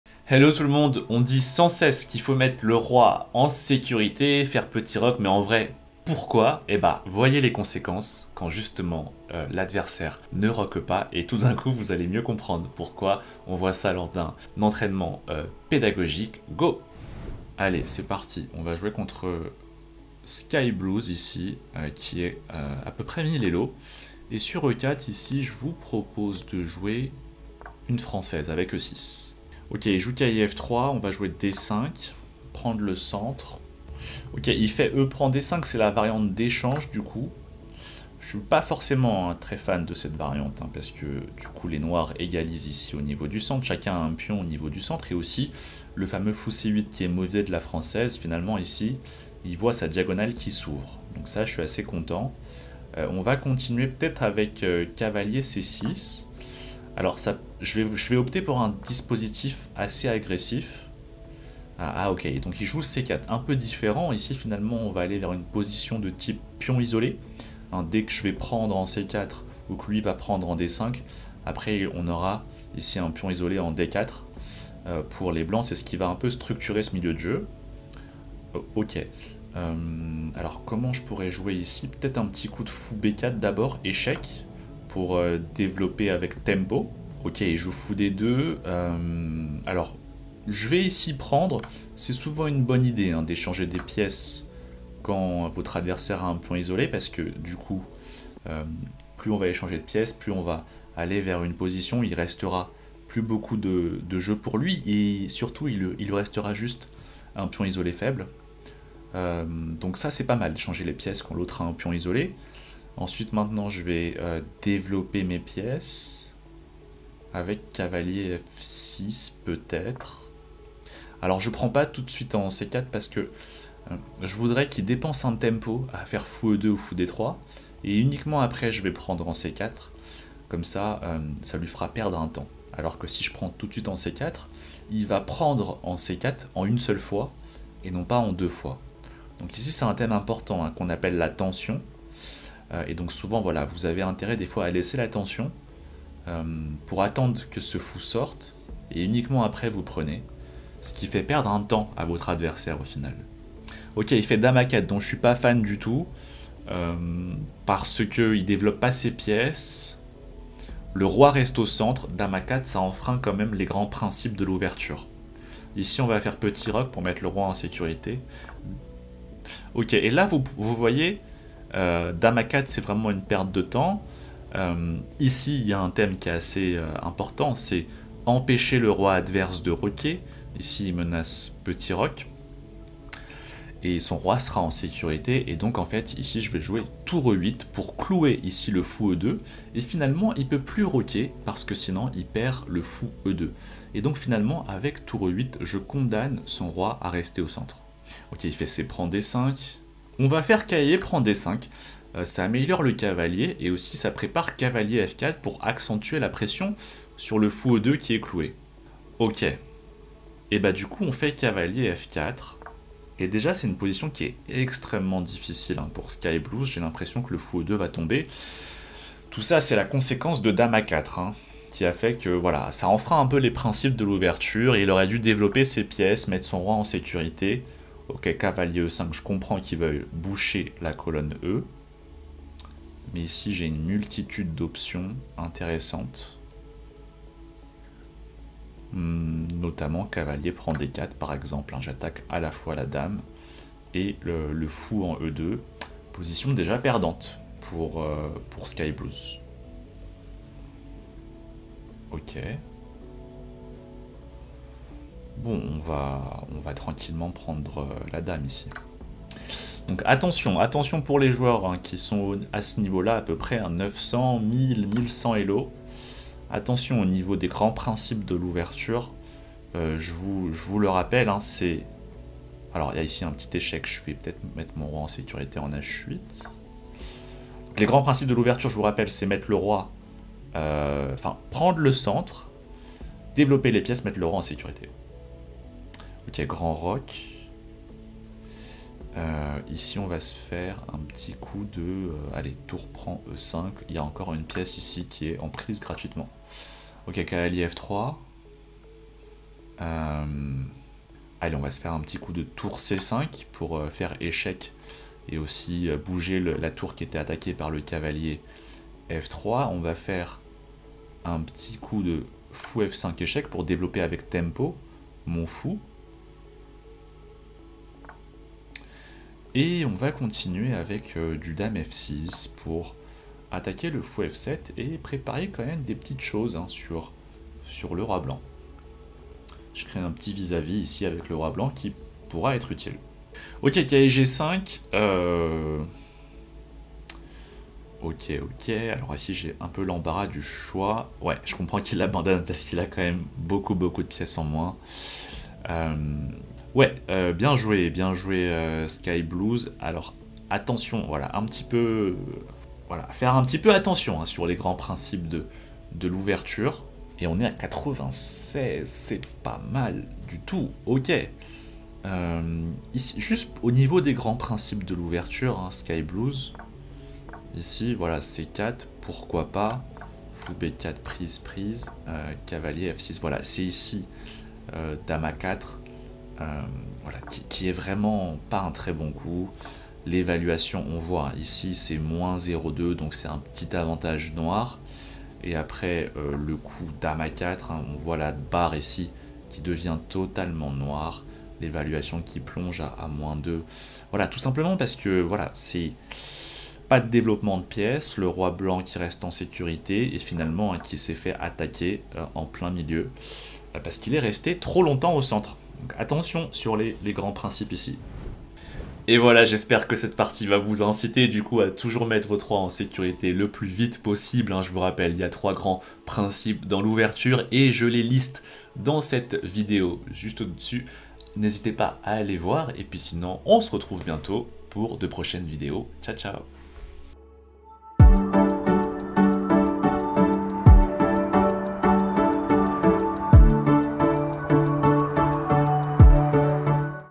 Ce qui se passe quand vous ne roquez pas...
Il existe trois grands principes dans l'ouverture dont celui-là : roquer vite pour mettre le roi en sécurité. Ca parait un peu catégorique comme conseil, mais alors qu'est ce qui se passe quand justement on ne le fait pas ? Réponse en image dans la vidéo !